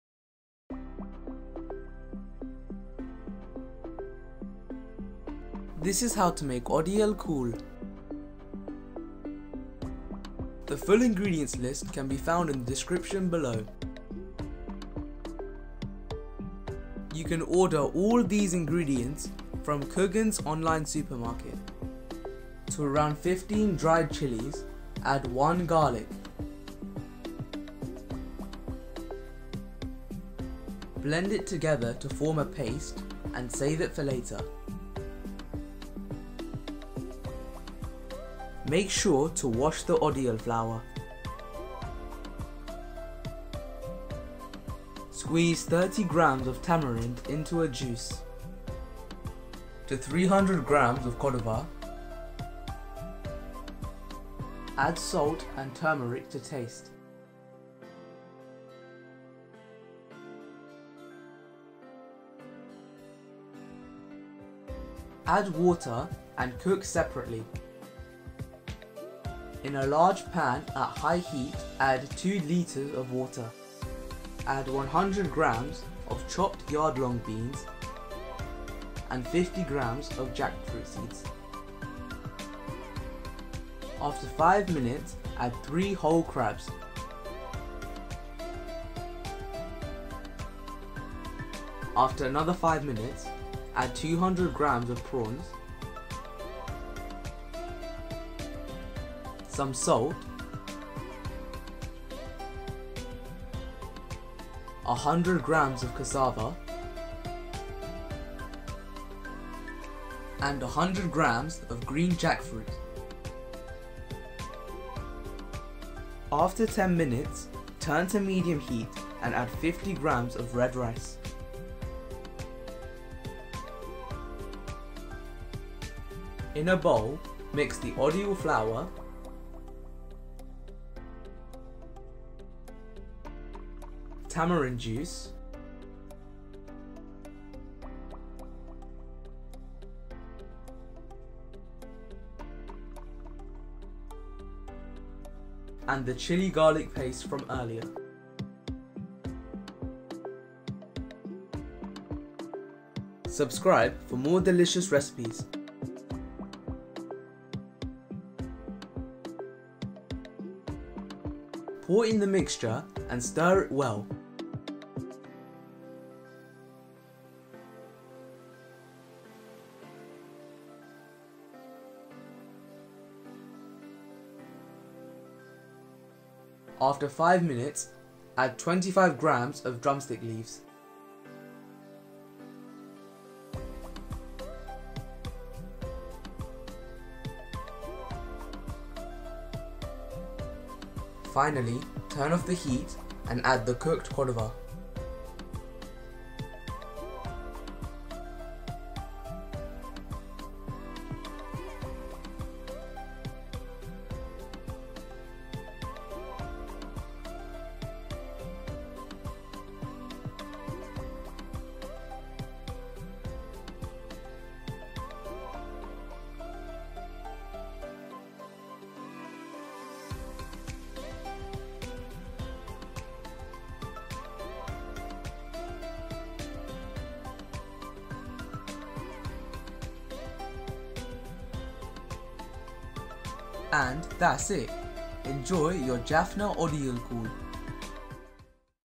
JAFFNA ODIYAL KOOL | How to make Spicy Odiyal Kool
Learn how to make a Jaffna traditional odiyal kool. Recipe for kool, a north Sri Lankan seafood dish that is perfect for the winter.

Ingredients:
100g odiyal flour
15 dried red chilli
1 garlic
100g cassava
300g fish koduva (sea bass)
3 whole crabs
200g prawns
100g yardlong beans
25g drumstick leaf
50g jackfruit seeds
100g green jackfruit
30g tamarind
50g brown boiled rice
Salt
Turmeric